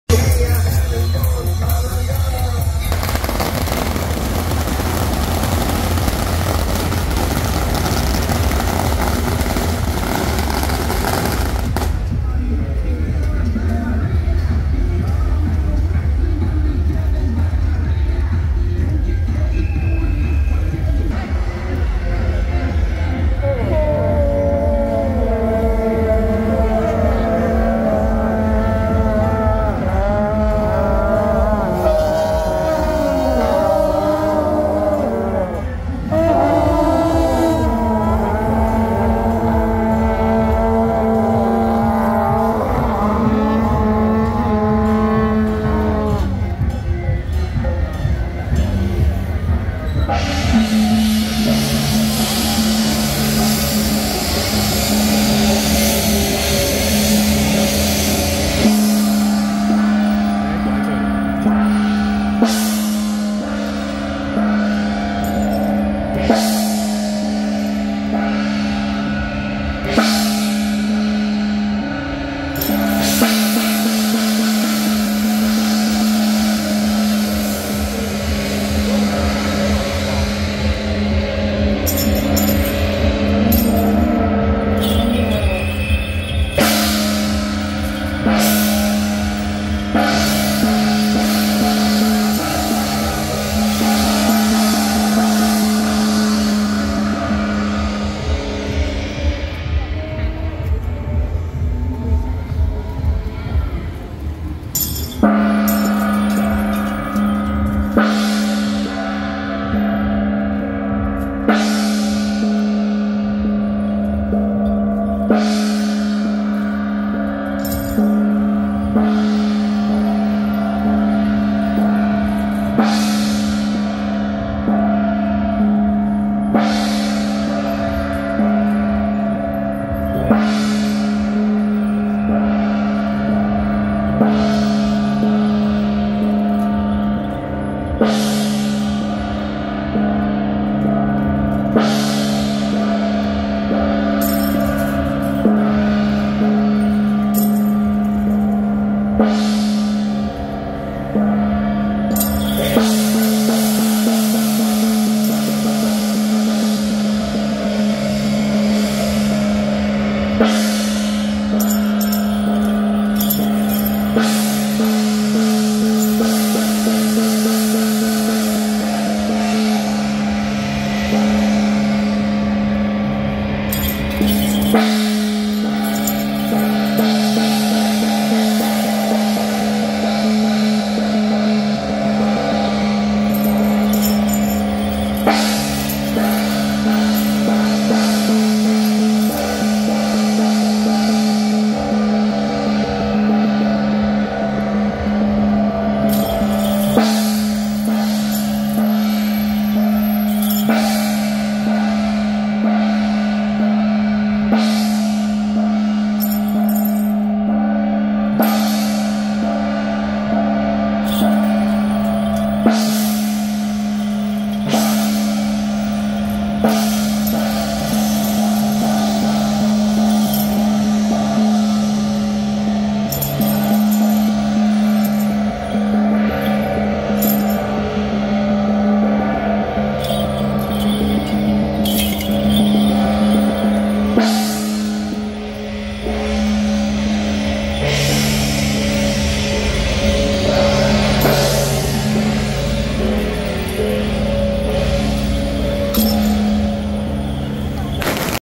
【內湖天福宮 邢天民俗藝術團-官將首】~112內湖玄聖宮真武大帝南巡進香三載圓科暨建宮12週年遶境
汐止九玄會-哨角團＆內湖天福宮 邢天民俗藝術團-官將首